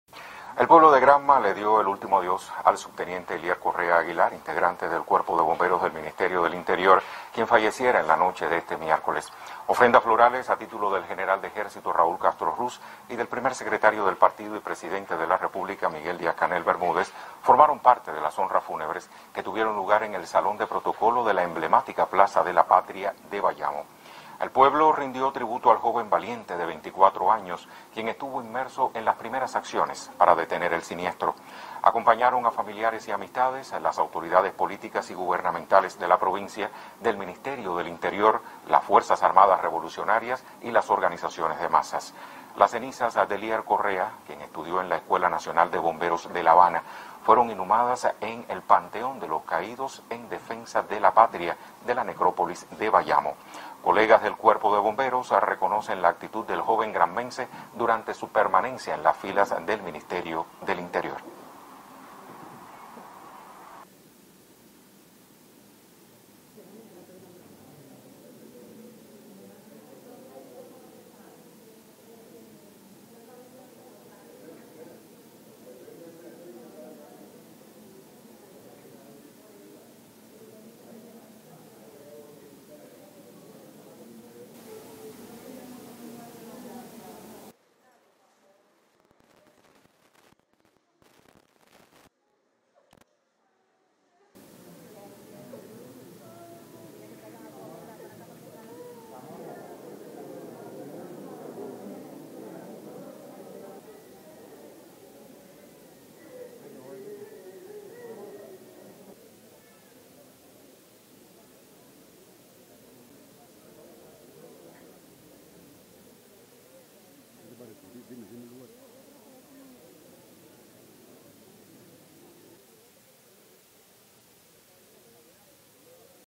Cuba rinde homenaje al joven bombero Elier Correa
Este jueves, en la Plaza de la Patria de Bayamo ―su ciudad natal― el pueblo de Cuba consternado, orgulloso y respetuoso, se unió al dolor de familiares, amigos y compañeros de labor de ElierCorrea, para darle el último adiós al bisoño combatiente del Ministerio del Interior (Minint), quien le había asegurado por teléfono a su madre Mirna: «mami, yo soy el primero en la fila, no tengo miedo».